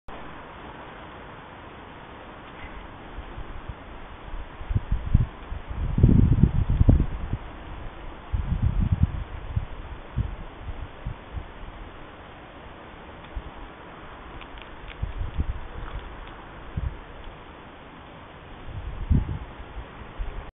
Hedgehogs fight
Two recently returned hogs in our garden having a bit of an altercation over the food bowl. Always seems to be the smaller one that beats up the larger one